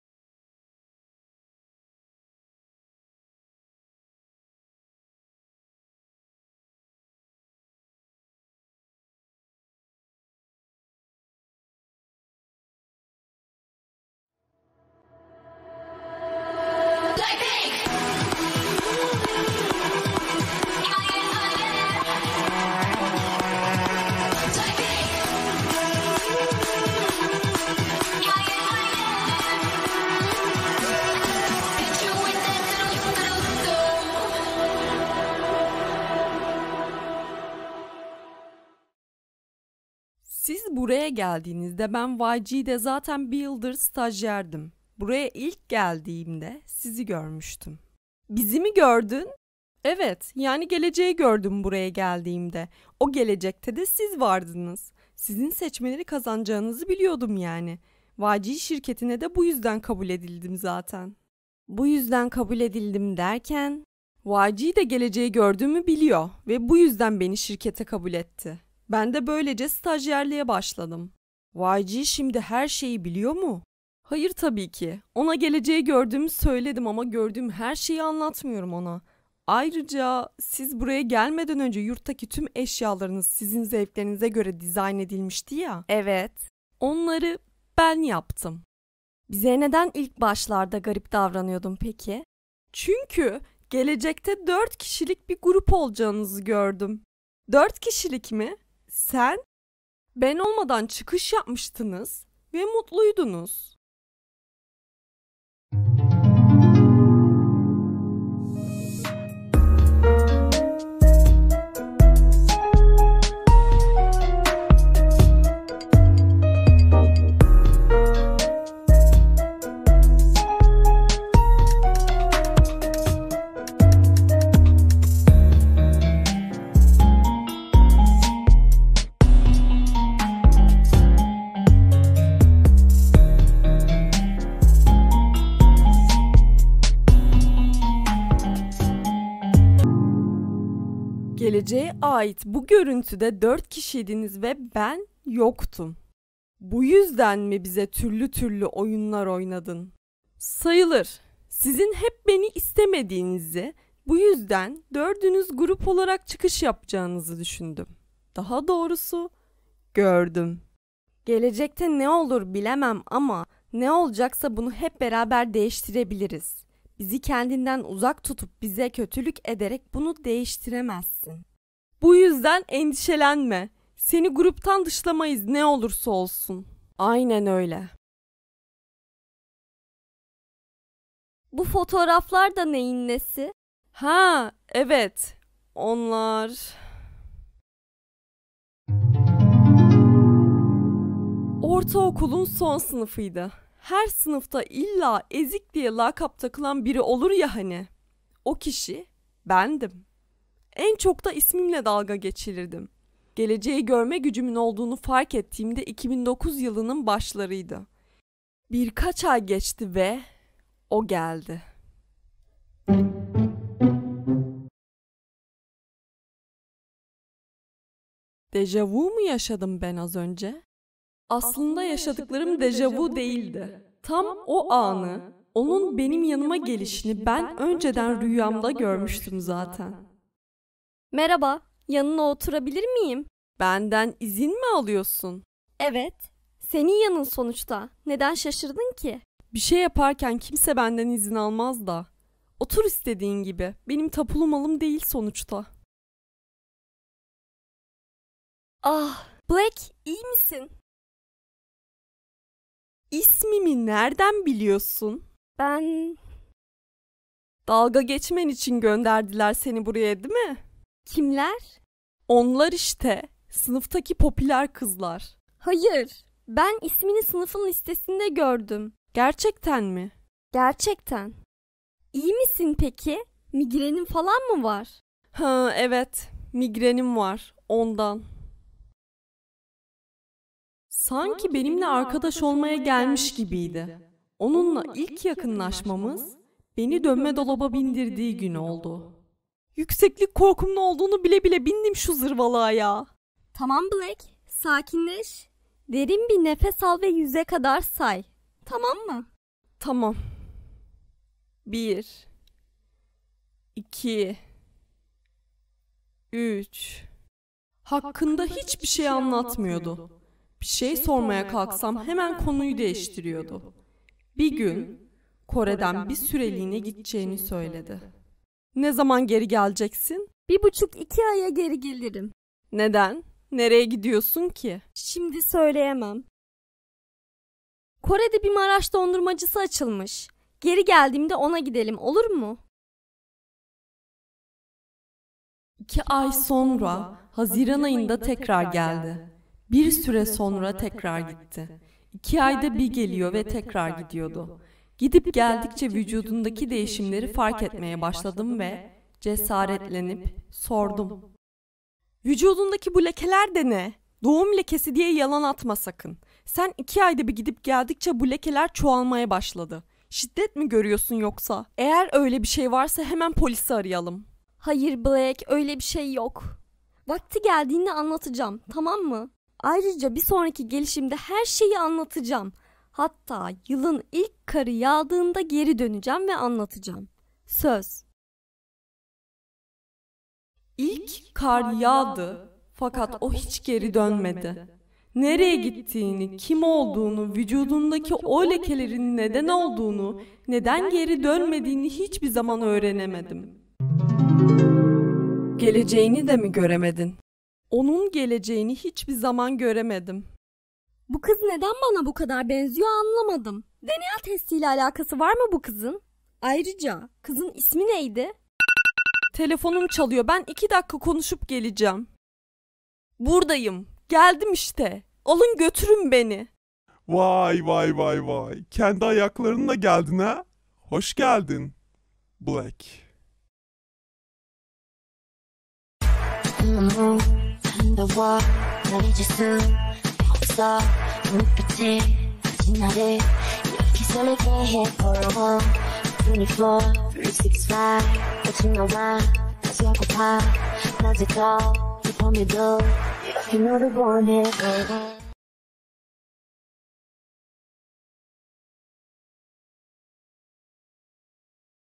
Söz | GAME OF BLACKPINK 12. Bölüm
Dünyaca ünlü BLACKPINK grubundan ilham alarak yaptığım GAME OF BLACKPINK çizgi dizisinin kurgusu, olayları, ek karakterleri, karakterlerin davranışları tamamen hayal ürünüdür. Kızların stajyerlik döneminden bağımsızdır. Ters bir düşünce güdülmemiştir.

Üzerinde çok emek verdiğim bu çizgi diziyi umarım severek izlersiniz. Keyifli seyirler! 🥳